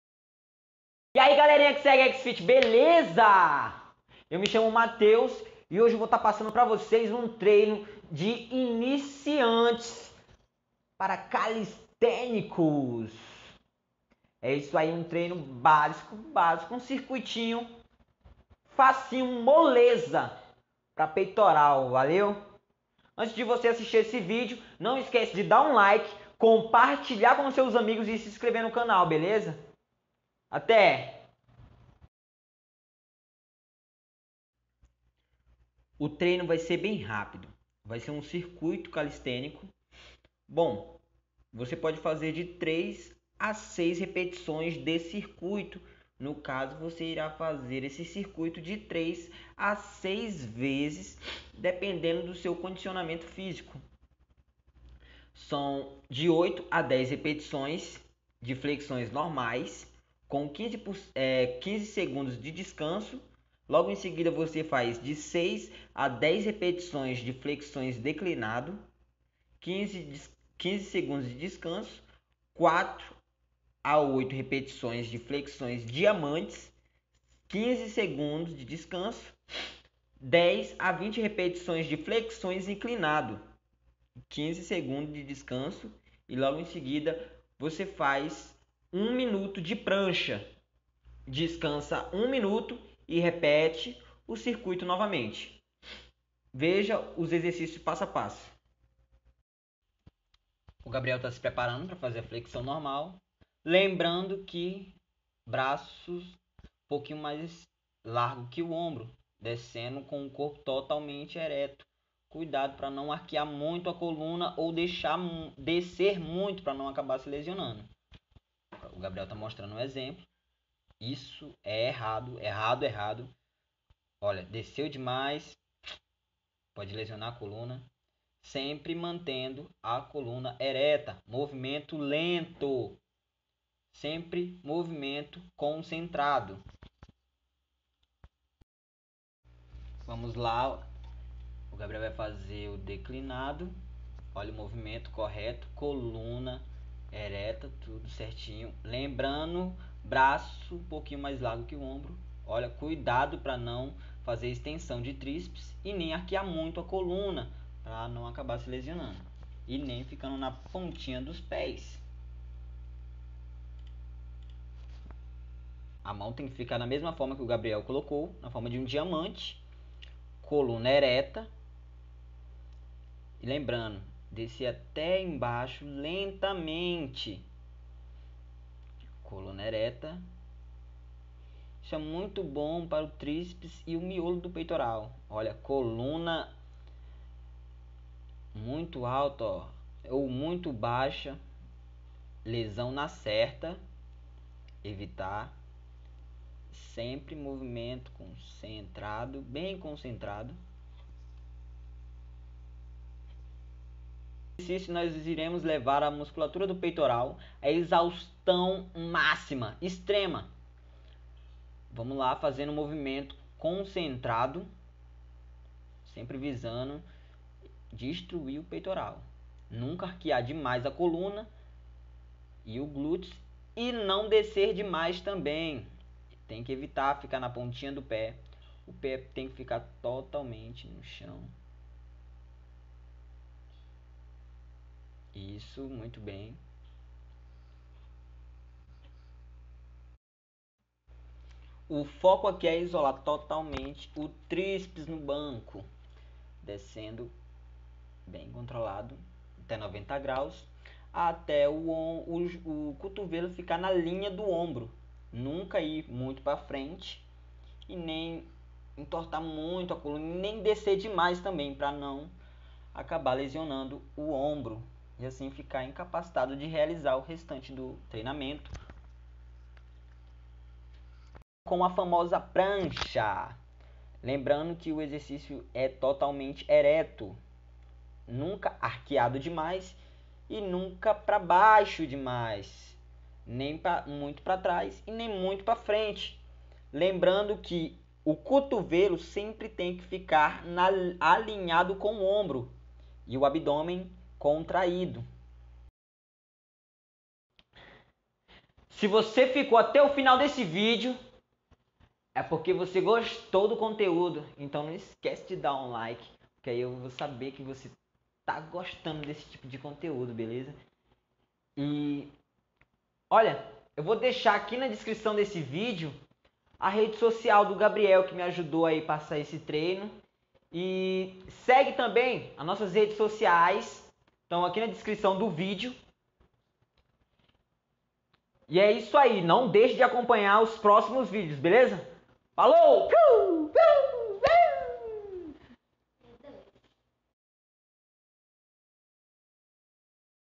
Rasgando o peitoral - Iniciantes - Calistenia
“O treinamento nos dá uma saída para as energias reprimidas criadas pelo estresse e tonifica, assim, o espírito, assim como as condições de exercitar o corpo”.

Nesse video, montei um treino totalmente exclusivo para quem deseja iniciar na calistenia, com um dos primeiros treinos de muitos que viram..

siga o Gabriel no instagran: gabrielmarque901

ACEITAMOS SUGESTÕES DE CONTEÚDOS.